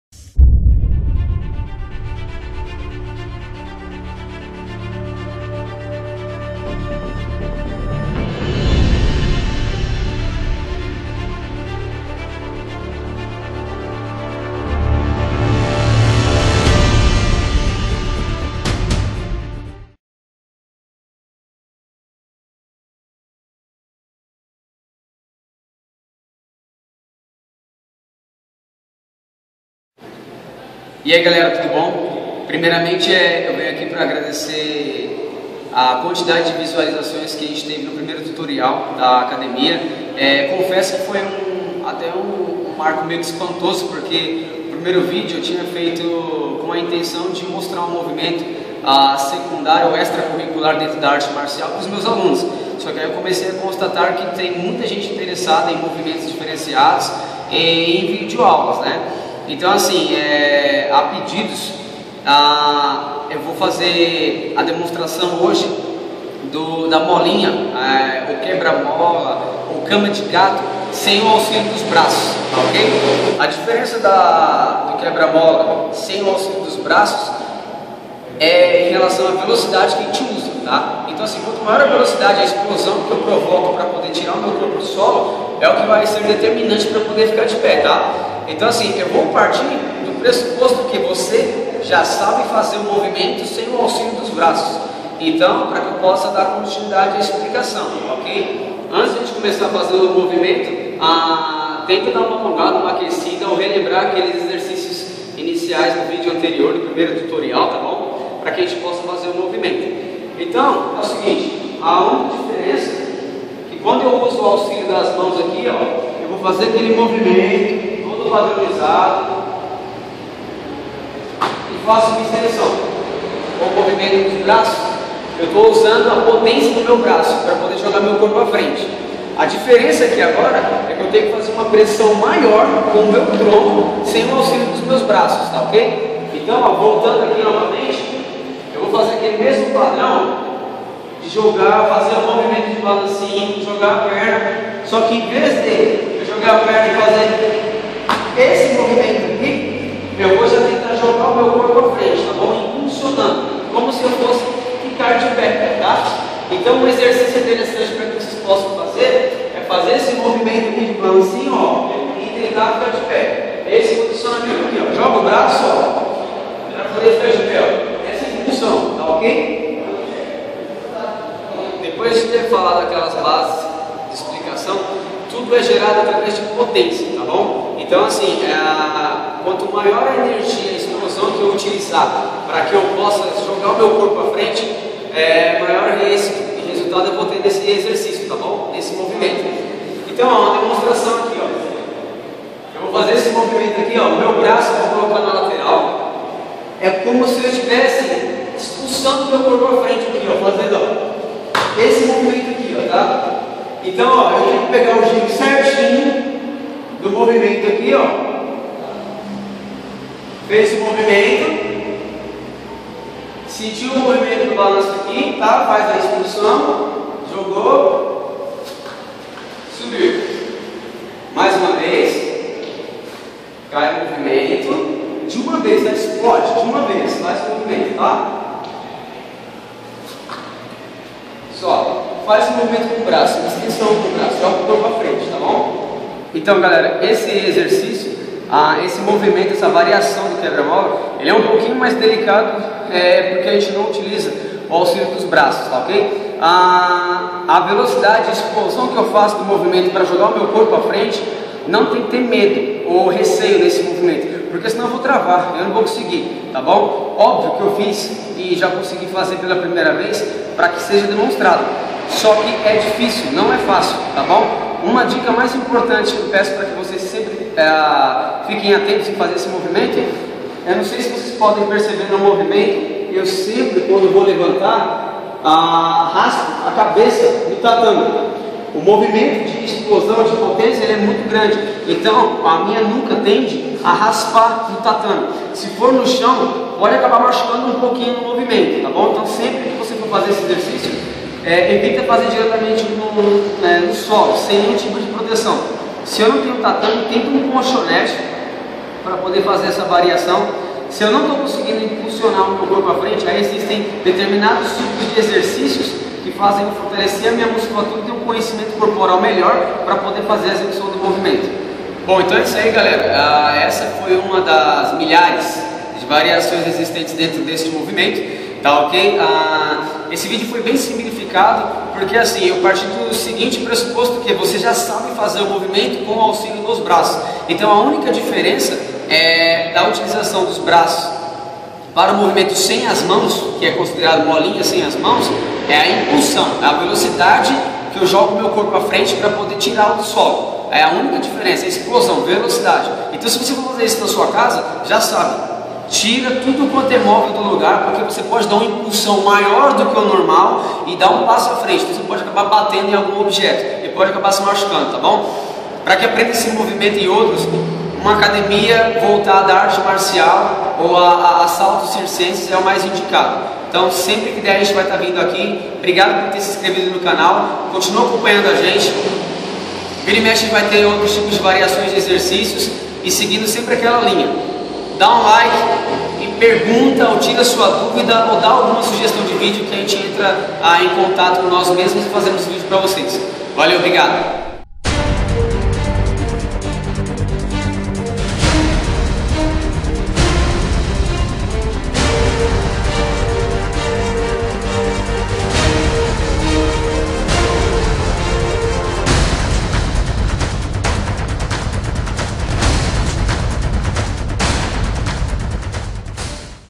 Vídeo Aula 02: Passo a passo do movimento "molinha" sem o auxílio das mãos
Vídeo aula 02: Como executar o movimento 'quebra mola", "molinha" ou "cama de gato" sem o auxílio das mãos.